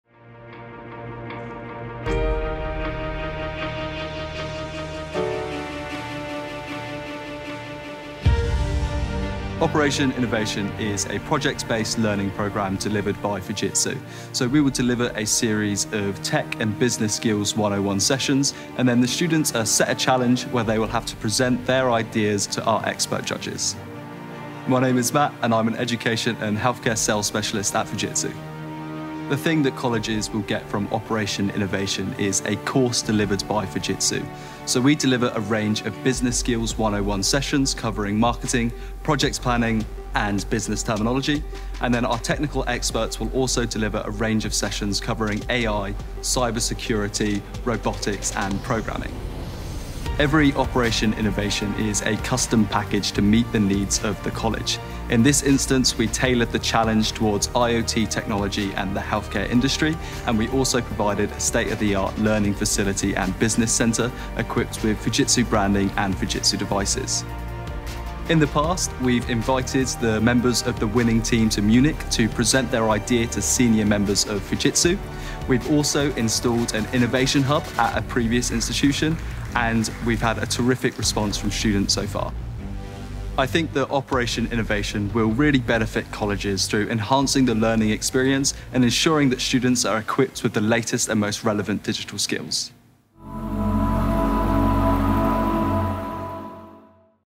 What is Operation Innovation?
Operation Innovation was established in 2016 to make Fujitsu's technical expertise more accessible to the next generation of bright minds, and to co-create the next big technology innovation. We have had many successful years of delivering the education program for 16-18-year-olds. A project-based learning program delivered by Fujitsu, who are on the cutting edge of technology innovation. It's about sharing industry expertise, supporting students in their STEM and business development, and fostering innovation. We want to help bring your brilliant ideas to life and tackle a real-world challenge or demand.